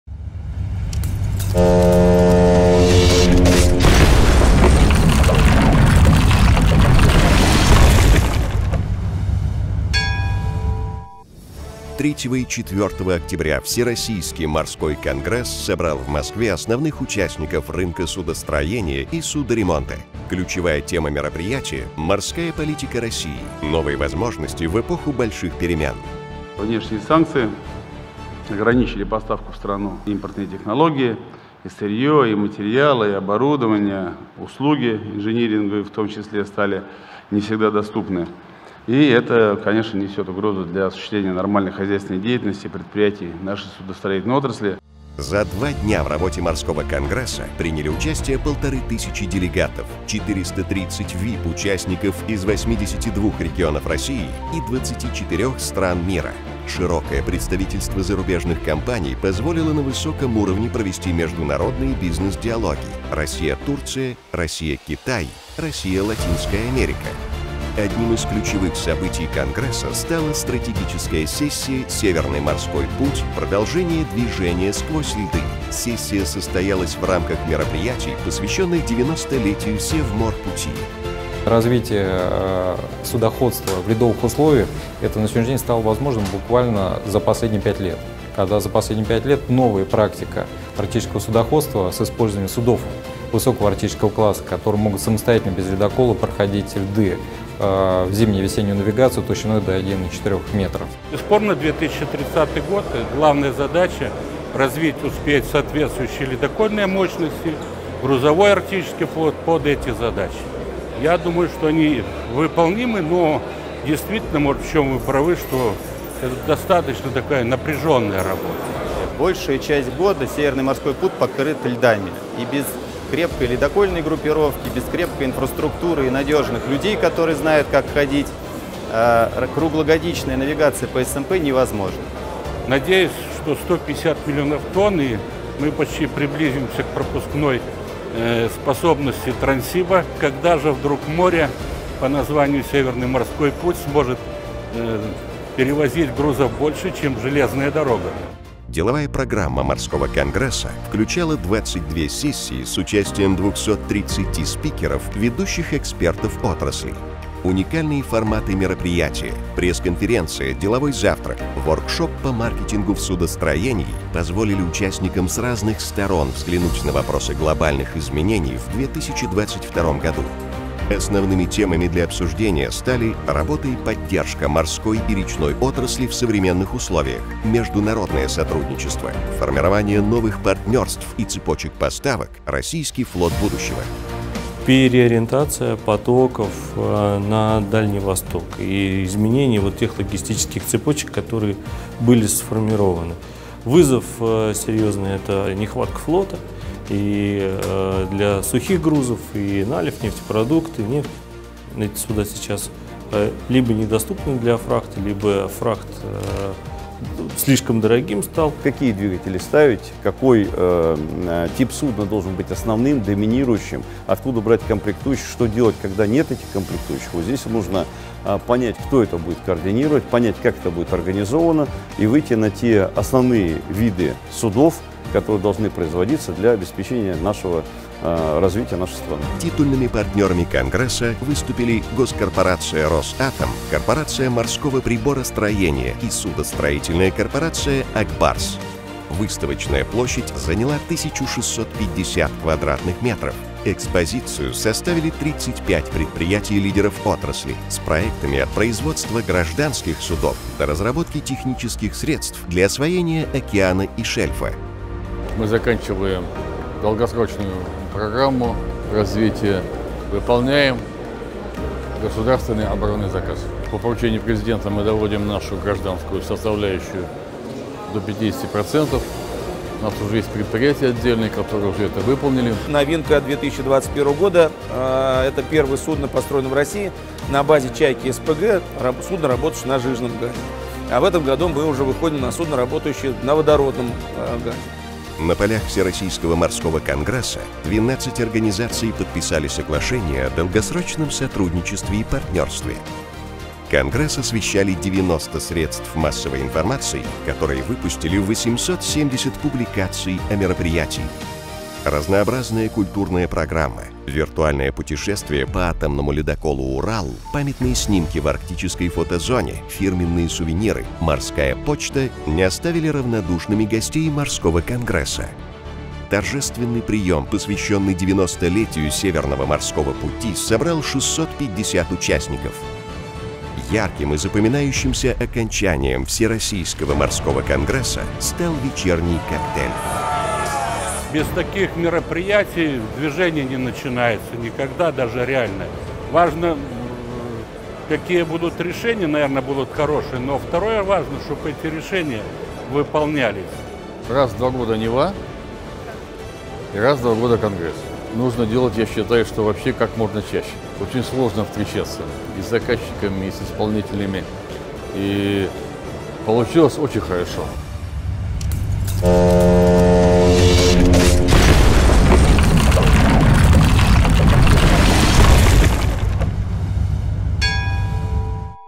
Отчетный видеофильм I Всероссийский Морской конгресс 2022
3 и 4 октября в Москве с успехом завершилось масштабное мероприятие — Всероссийский Морской конгресс, организованный при поддержке Министерства промышленности и торговли РФ и титульных партнеров: Госкорпорации «Росатом», Корпорации Морского Приборостроения и Судостроительной корпорации «Ак Барс».

Всего площадку мероприятия посетили 1500 уникальных участников.